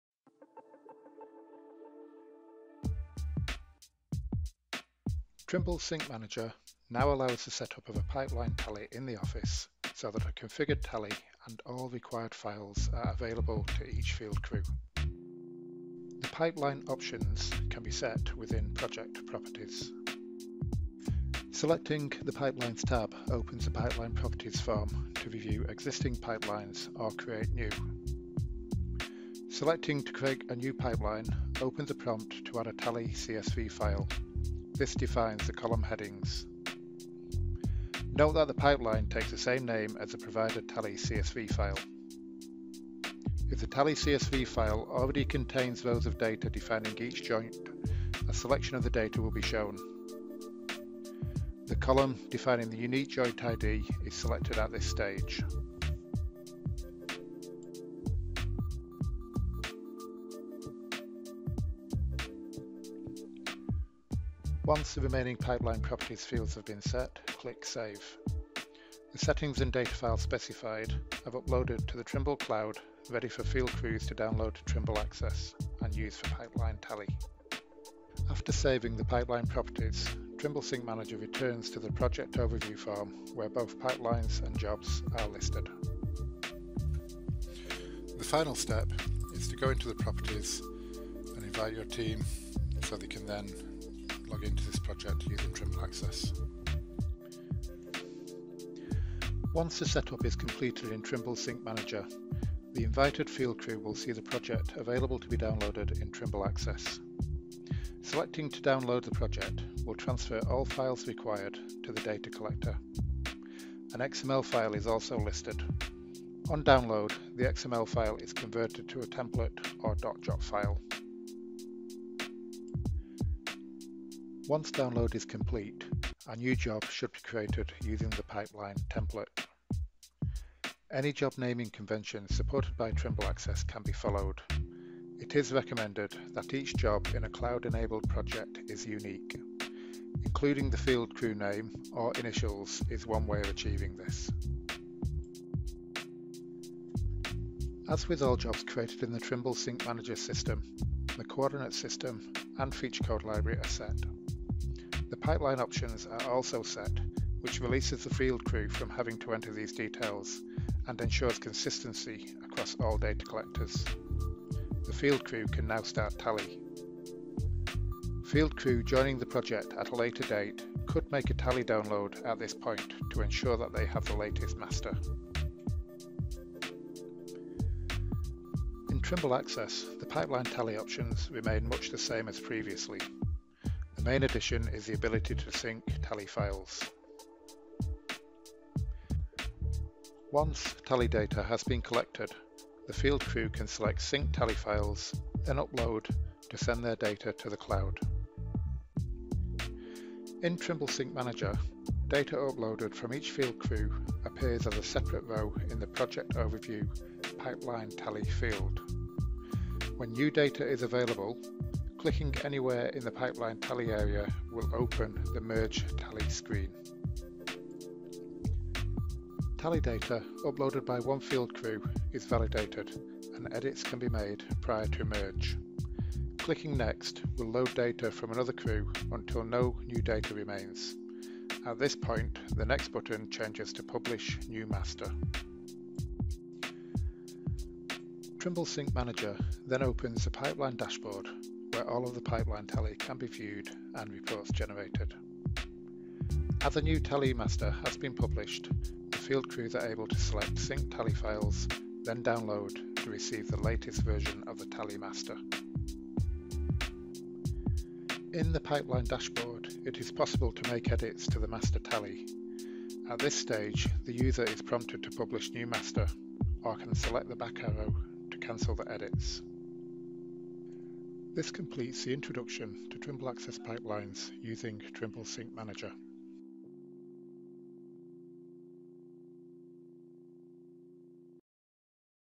Introduction to Trimble Sync Manager for Trimble Access Pipelines
An introduction to using Trimble Sync Manager to set up a pipeline tally for your pipeline project. On download of Trimble Access 2019.10 the tally is ready for use with Trimble Access Pipelines.
Cloud-based data management allows multiple field crews and the office to stay in sync.